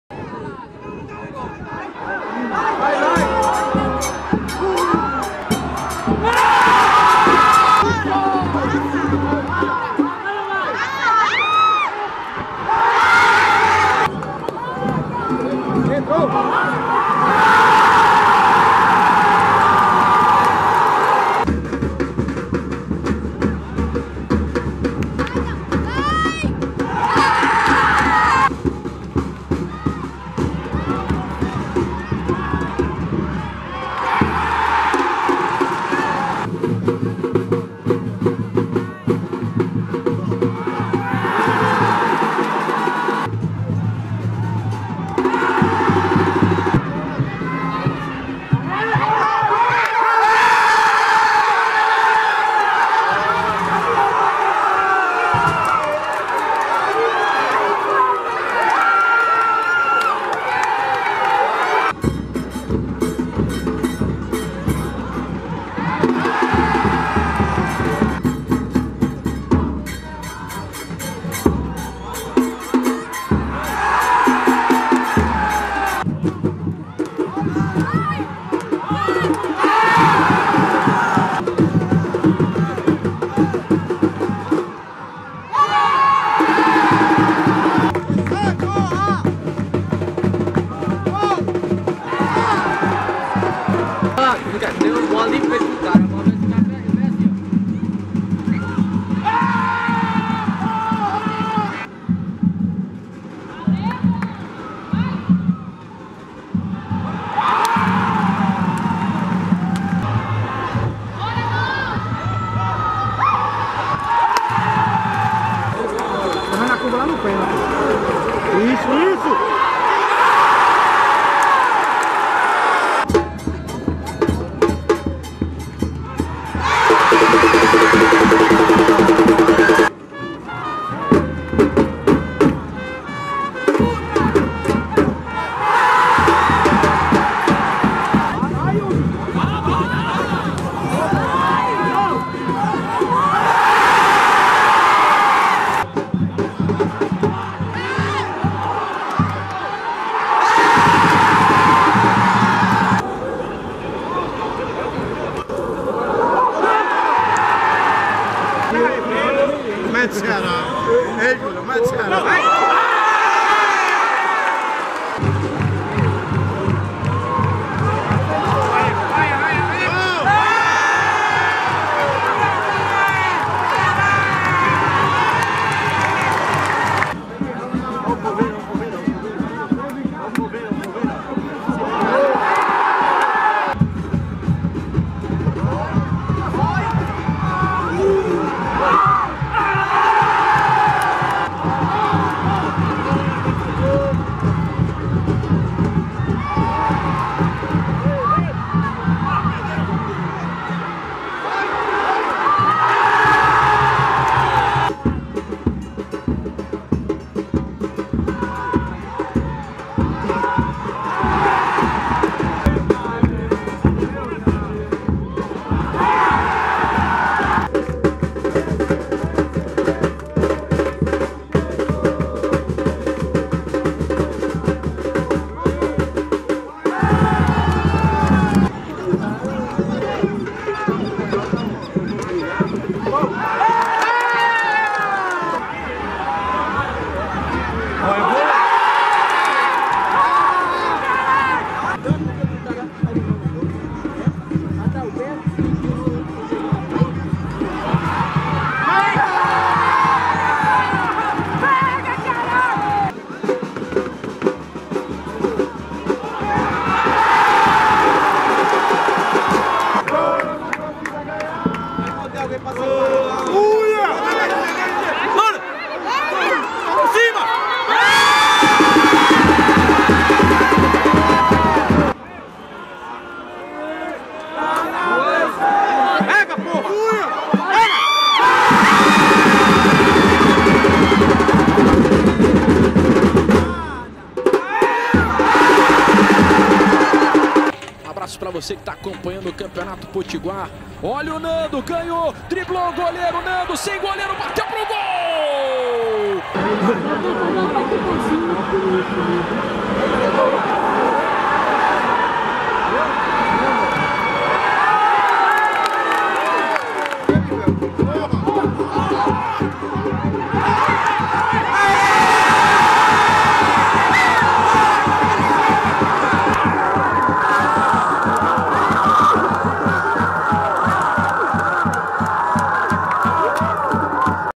ESPECIAL ESTADUAL 2017
Todos os gols que levaram o Mais Querido ao seu 54º título do Campeonato Potiguar!